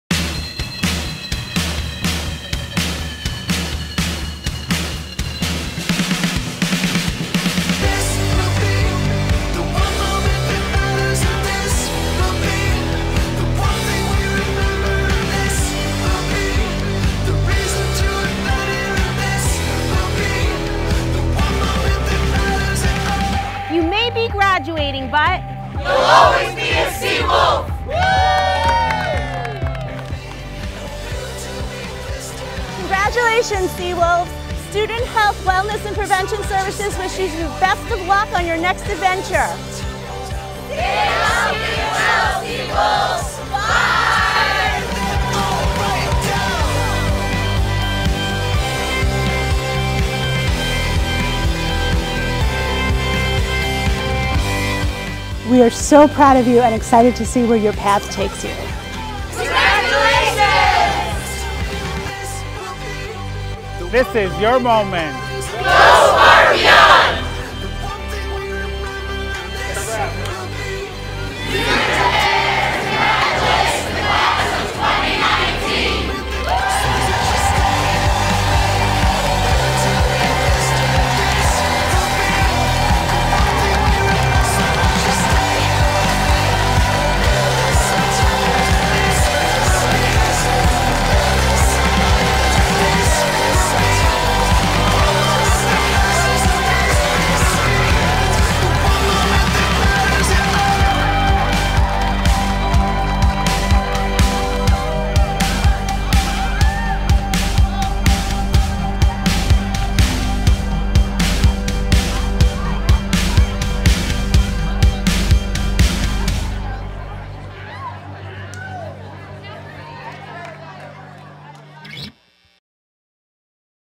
The One Moment... Congratulations Class of 2019!
The Division of Student Affairs wishes our grads the best of luck in this new phase of their lives. We're so proud of you!